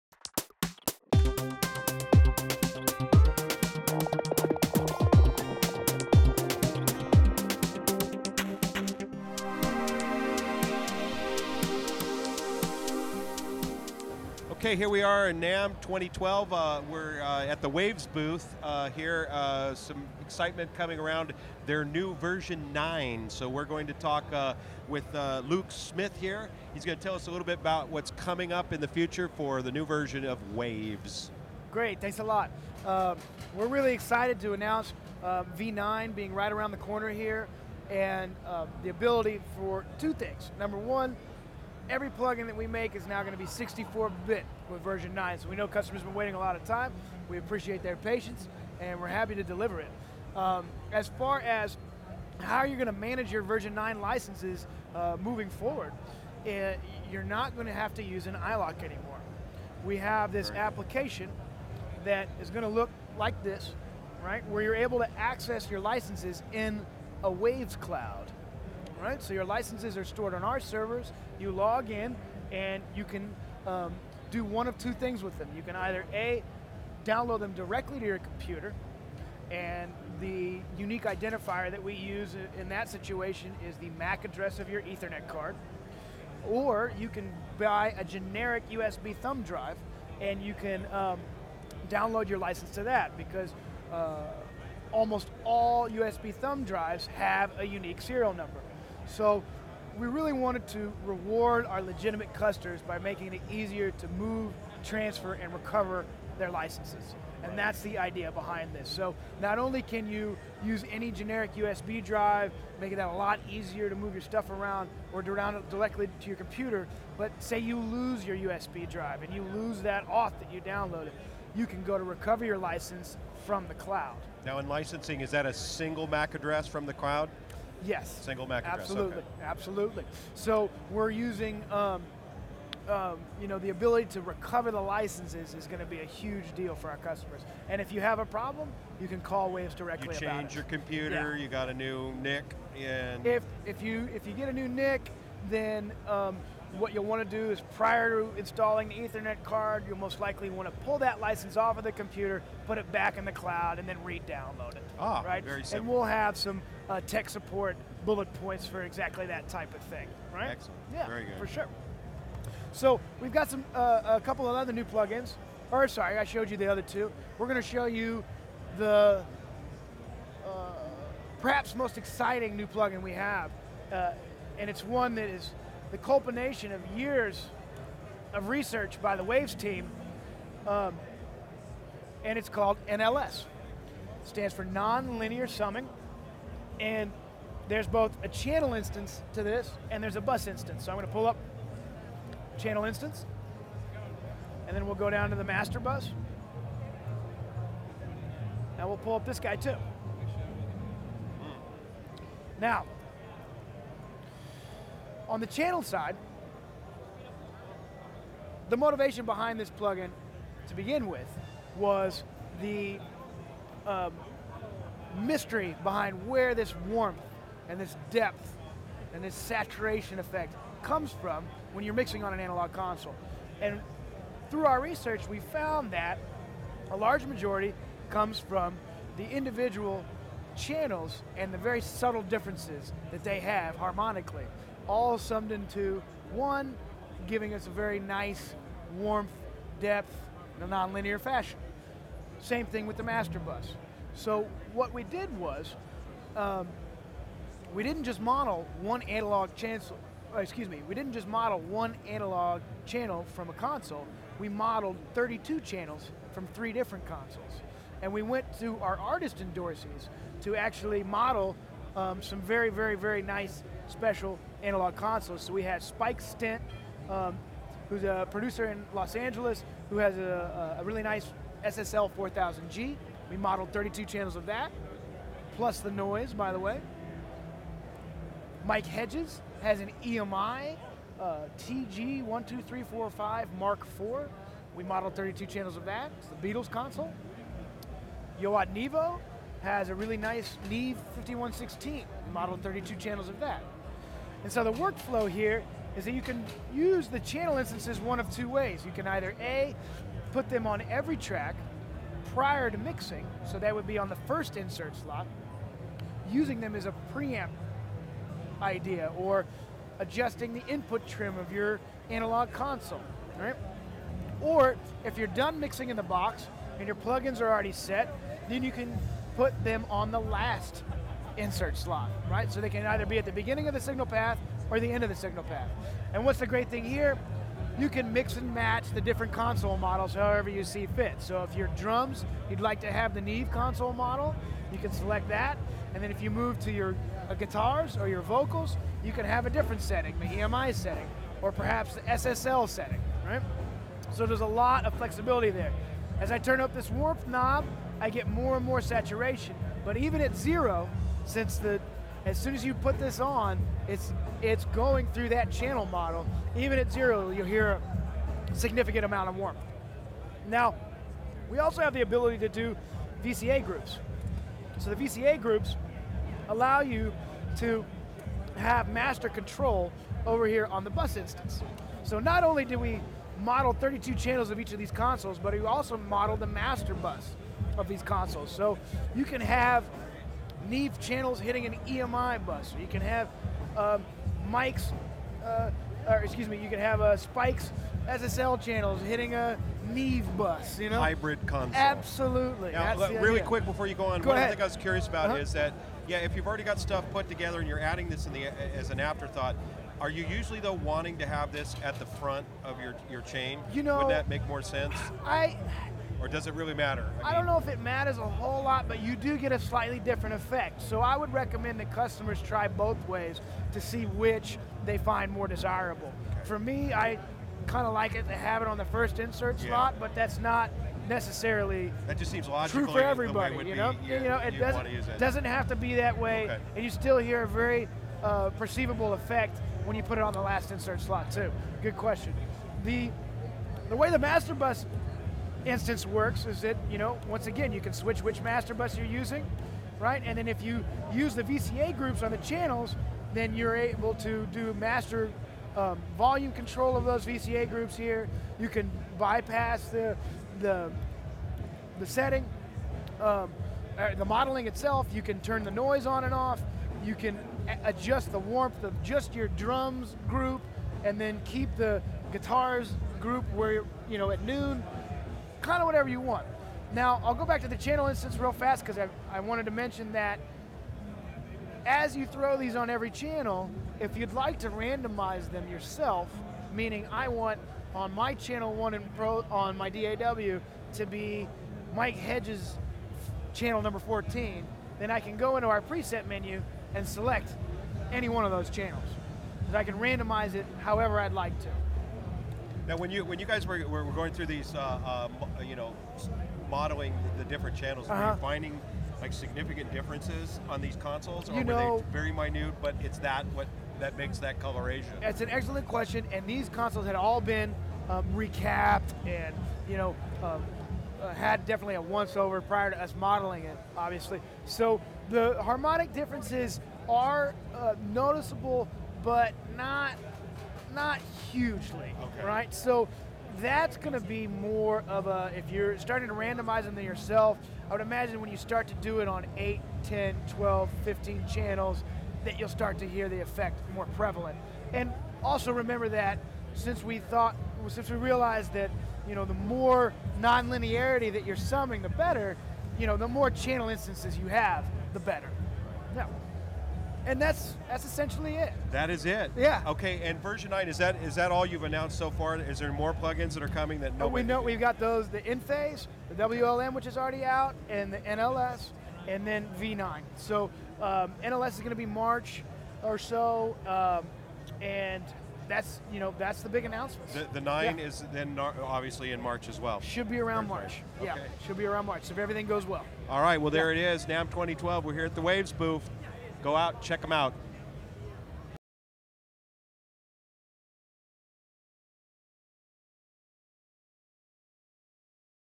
Waves NLS Summing Plugin and 64 Bit Version 9 - Namm 2012
Waves shows us their new plugins NLS Non Linear Summing and Ver 9 of their software which is going 64 bit in March 2012. They modeled 32 channels from 3 different Analog consoles. Spike Stent SSL 4000g, Mike Hedges EMI TG 12345 Mk IV Beatles Console, Yoad Nevo Neve 5116.